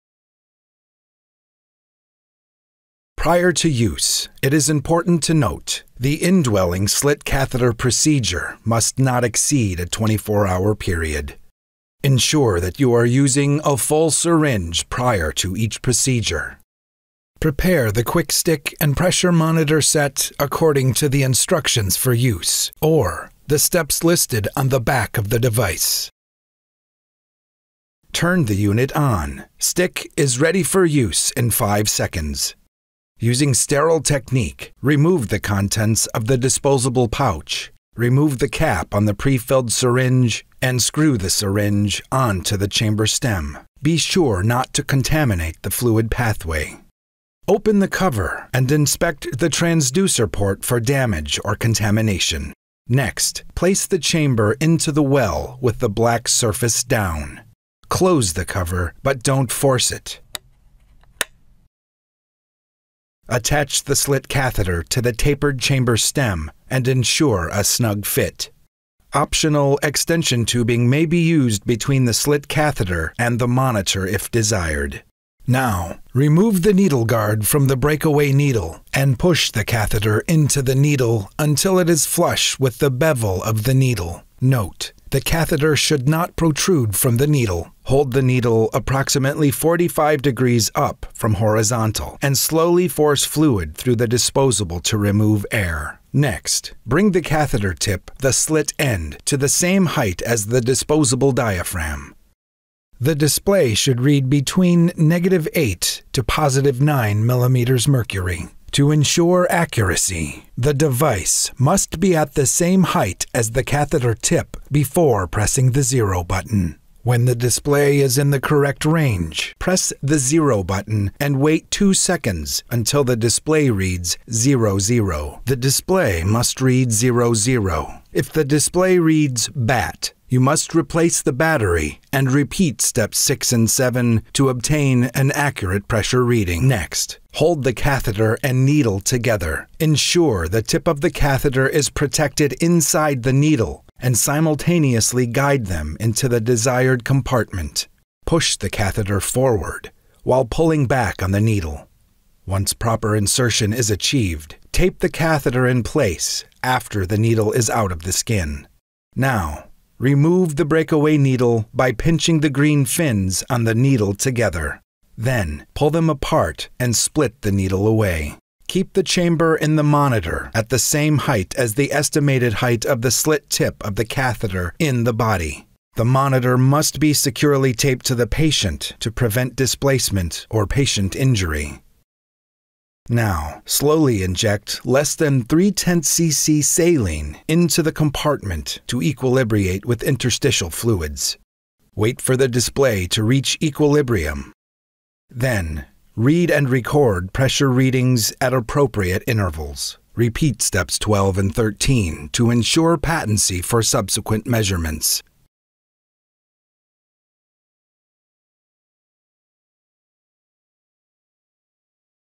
Measure Compartment Pressure - STIC (Indwelling Method) Instructions
STIC Intra-Compartmental Pressure Monitor System is a hand-held monitor for quick and accurate measurement of compartmental pressure. The "Indwelling Method" is a continuous monitoring testing application.

What is compartment syndrome?
Compartment syndrome can develop in patients with palpable pulse, acceptable color, and normal sensory examinations. It should be suspected in any extremity that has sustained high-energy soft tissue or bone trauma, arterial disruption, a burn, or limb compression. The one factor that must be present in compartment syndrome is increased tissue pressure.